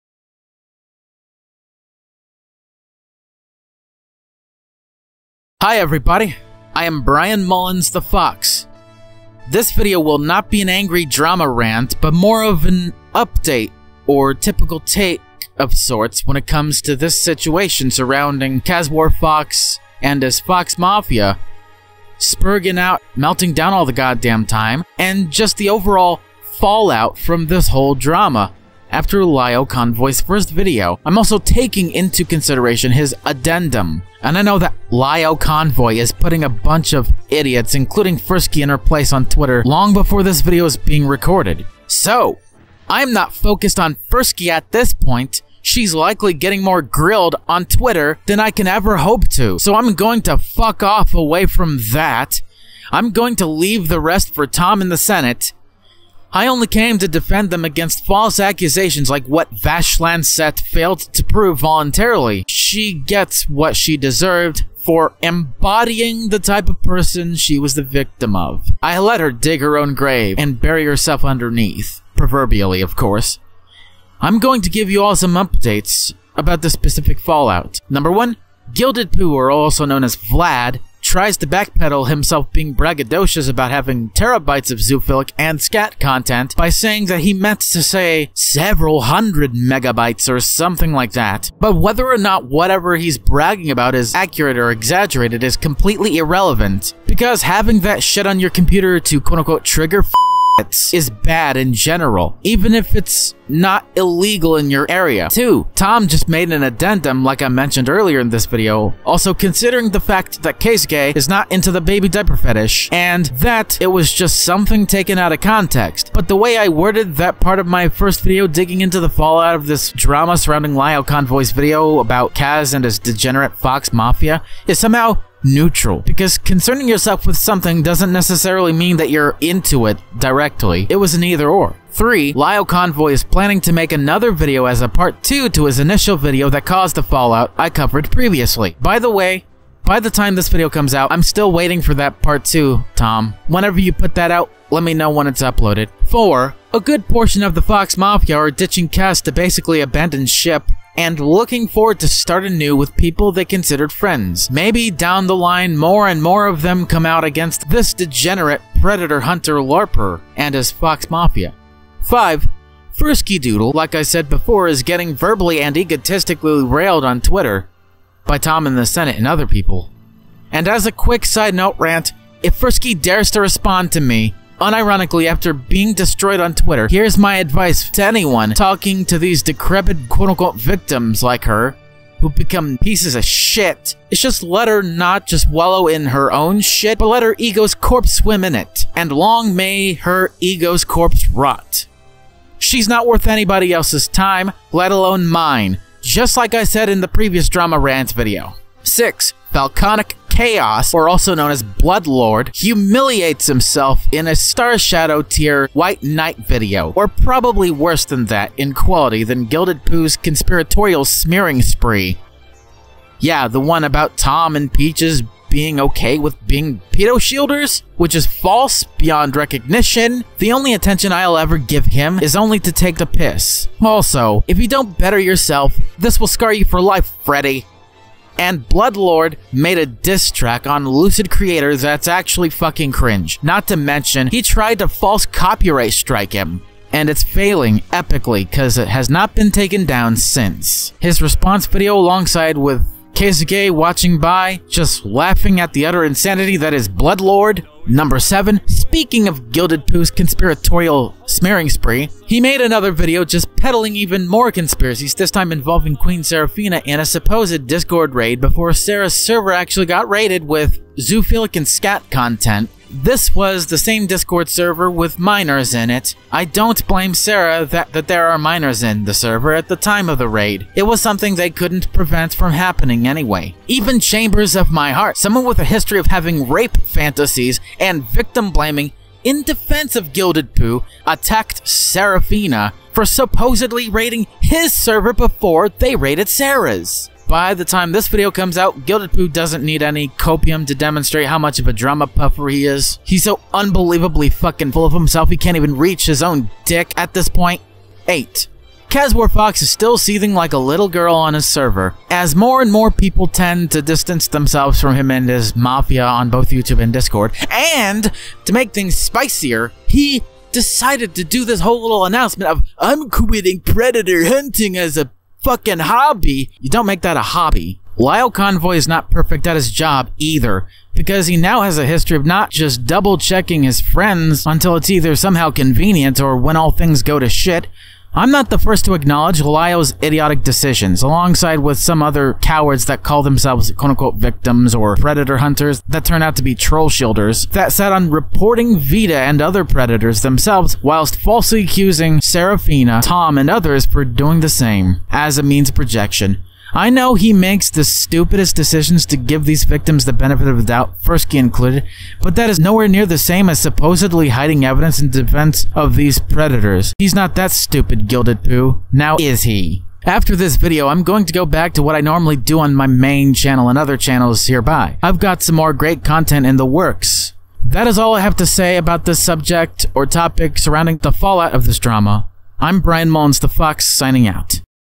My Final Video About The Fallout Of Caswarfox & The 'Fox Mafia,' Updates, And Clarifications - Rant!
Here is my final video on the matter, still waiting for Lio's part 2.